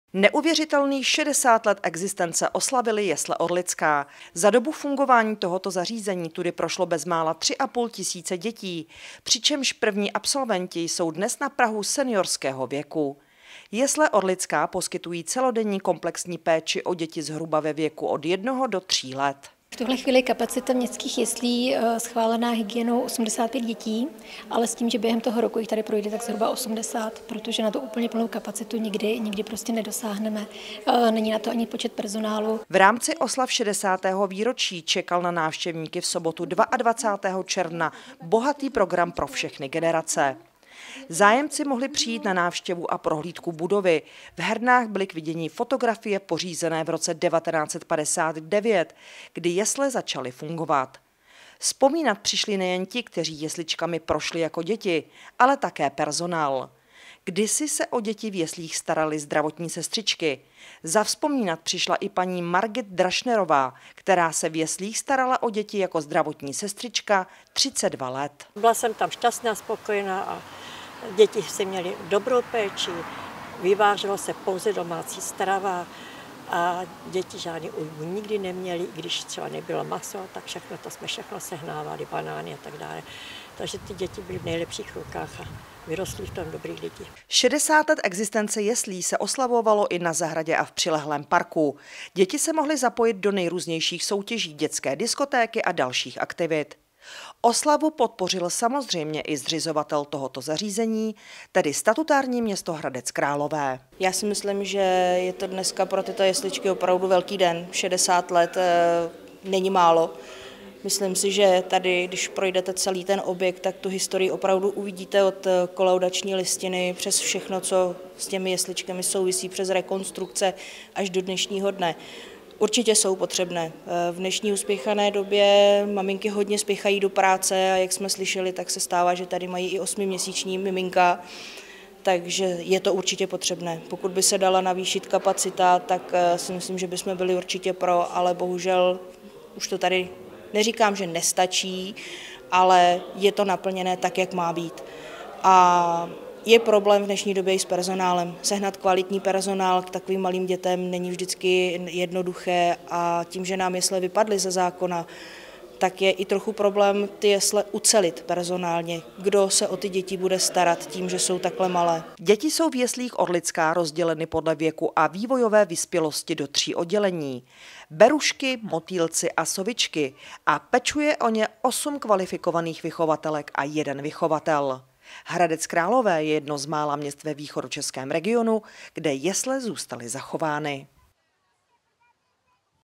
Městské jesle oslavily šedesát let existence
Neuvěřitelných 60 let existence oslavily Jesle Orlická. Za dobu fungování tohoto zařízení tudy prošlo bezmála tři a půl tisíce dětí, přičemž první absolventi jsou dnes na prahu seniorského věku. Jesle Orlická poskytují celodenní komplexní péči o děti zpravidla ve věku od jednoho do tří let.